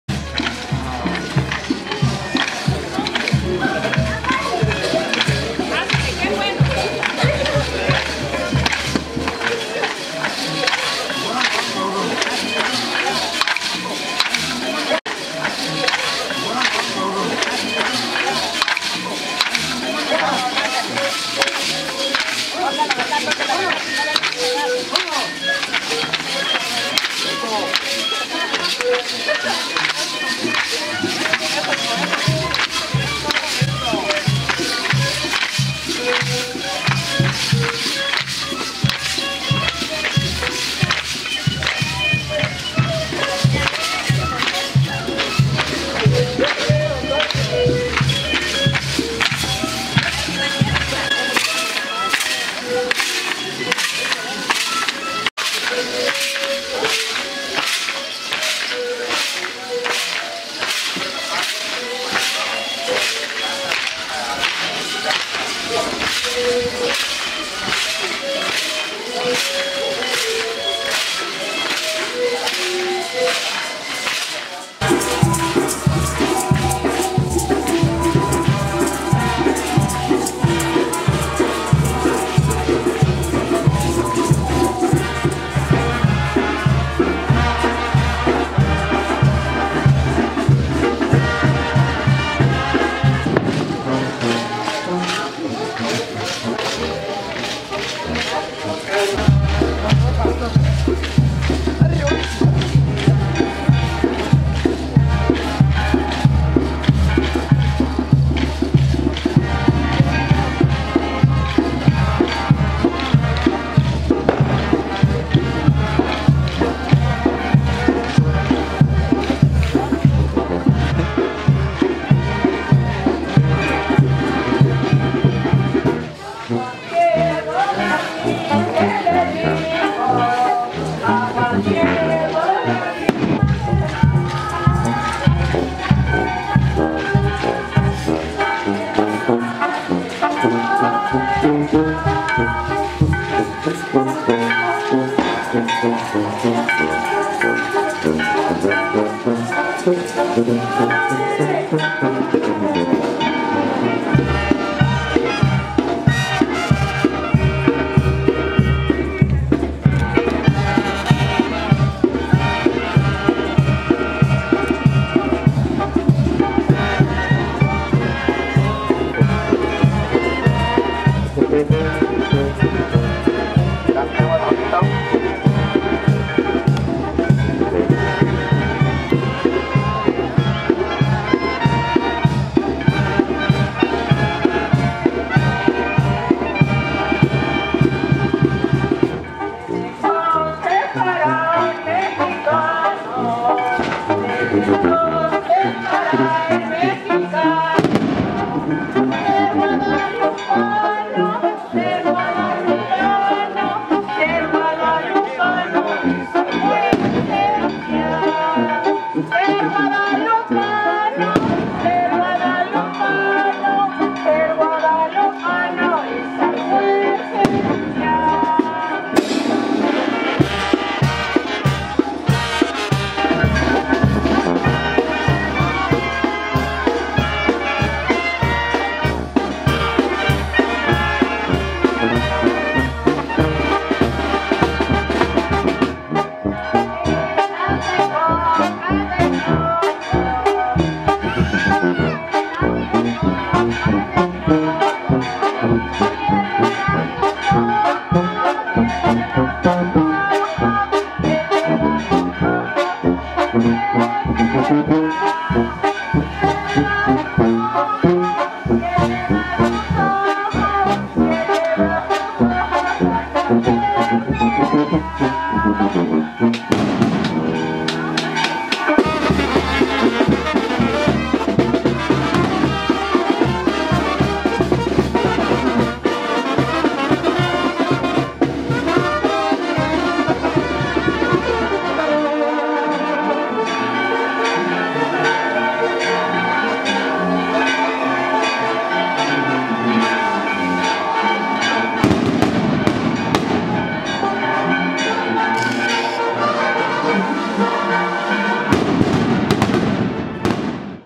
Peregrinación 2023 Puente de Camotlan, Nayarit [Fiestas Patronales] - 12 de Enero
Honor a Nuestra Senora de Guadalupe, Puente de Camotlan, La Yesca, Nayarit.

12 de Enero 2023.